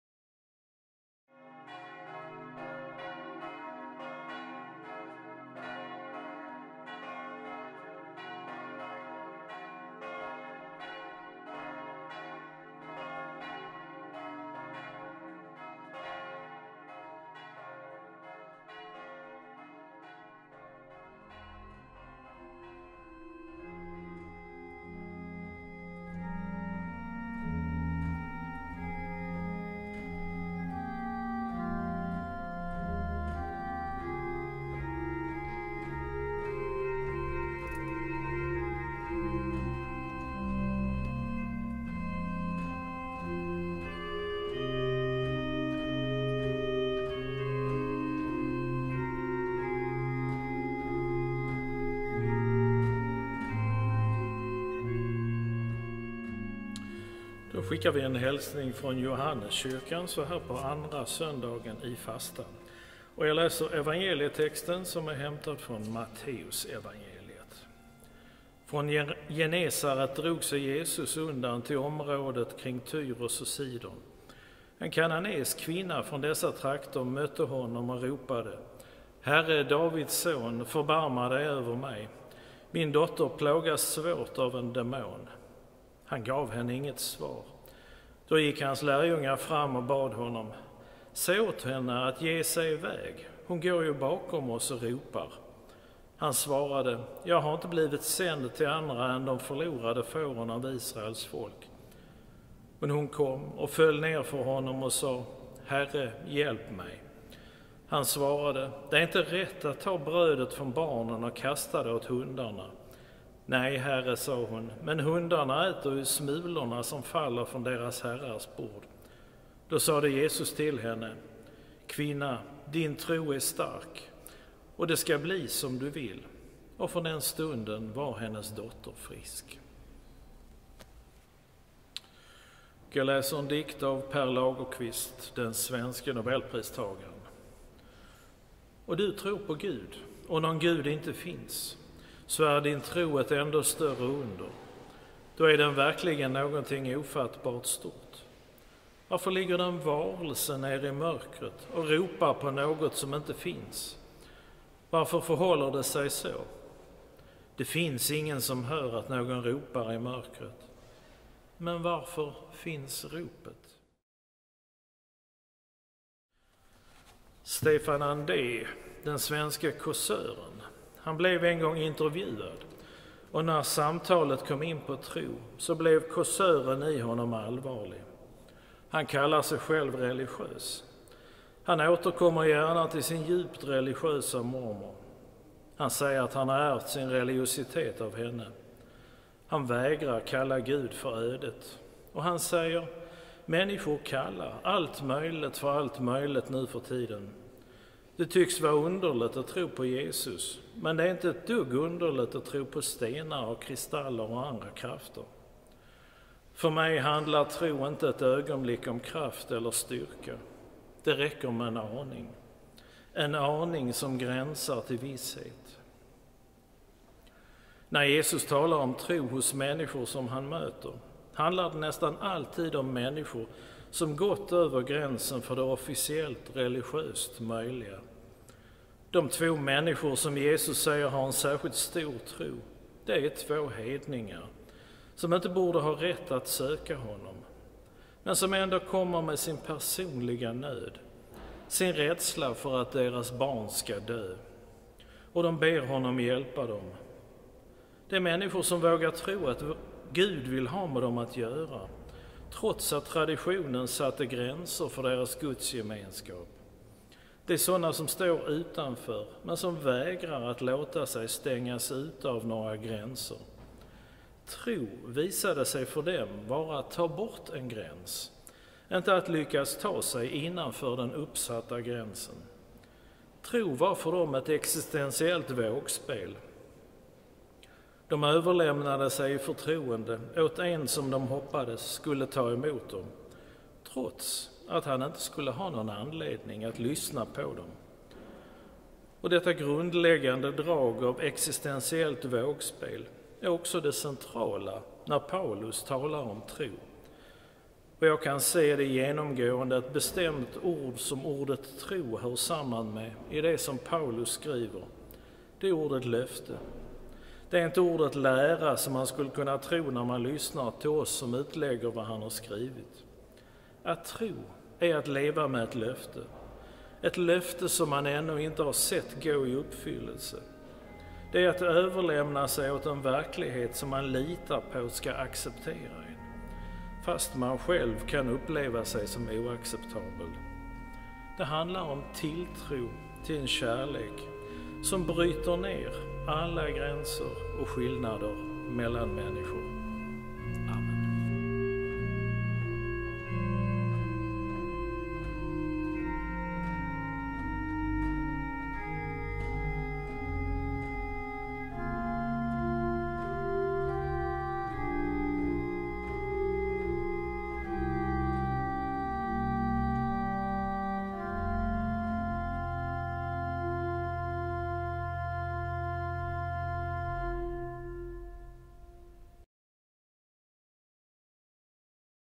Helgmålsbön från S:t Johannes kyrka - Andra söndagen i fastan
Johnny Wegestål, präst.
Christian Schultze, organist.